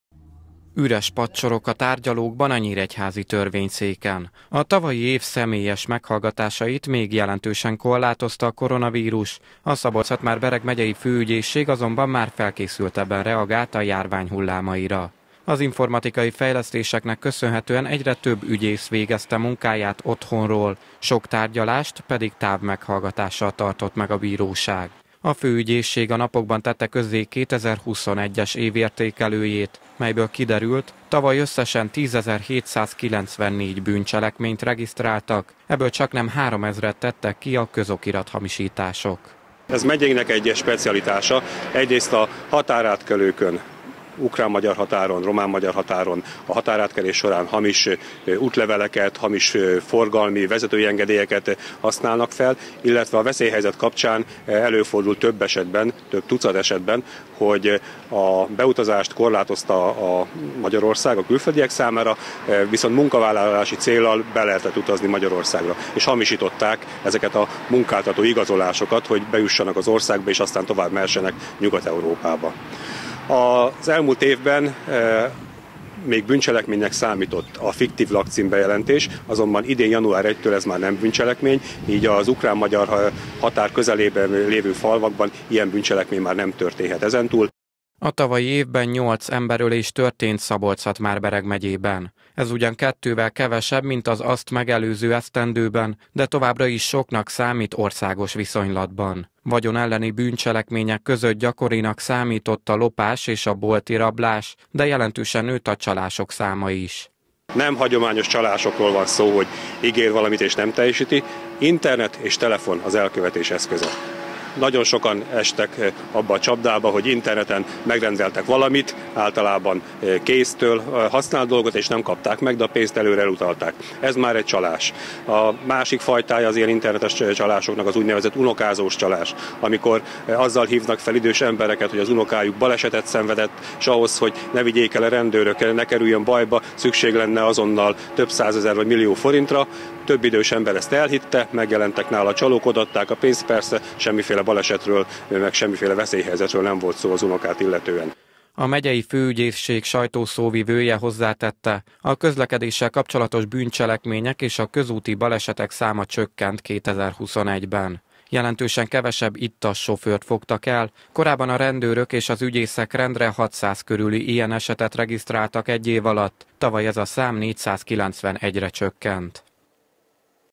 Egyre több bűncselekmény
Jelentősen, több mint öt százalékkal emelkedett tavaly a bűncselekmények száma Szabolcs-Szatmár-Bereg megyében 2020-hoz képest – ez derült ki a megyei Főügyészség éves jelentéséből. Tavaly nyolc emberölés történt, ami országos viszonylatban továbbra is soknak számít. Nagymértékben nőtt emellett a közokirat-hamisítások és a csalások száma, ittas sofőrből azonban lényegesen kevesebbet fogtak el, mint korábban.